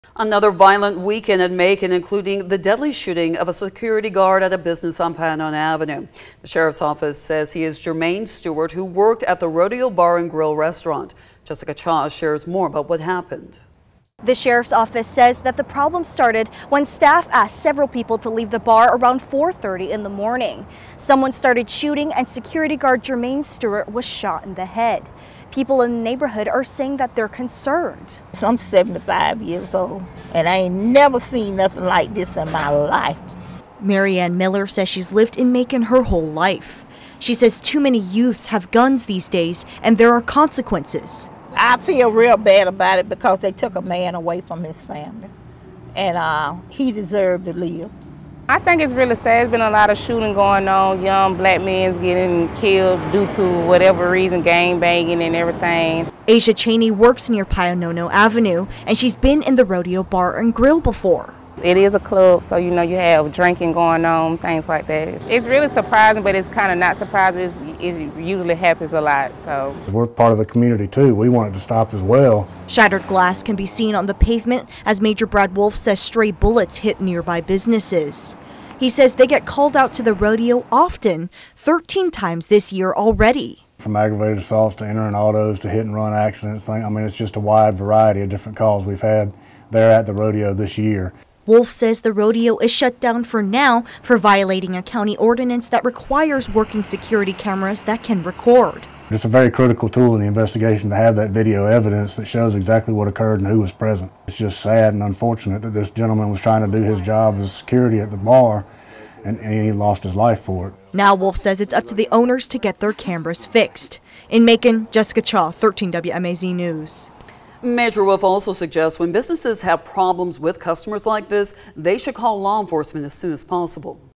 Bibb deputies investigating after customer shoots security guard at Macon bar
It's just sad that this gentleman was just trying to do his job as security at the bar and he lost his life over it,” Major Brad Wolfe explains.

13WMAZ News is the #1 station for news and weather in the Central Georgia area, serving people in Dublin, Macon, Milledgeville and Warner Robins.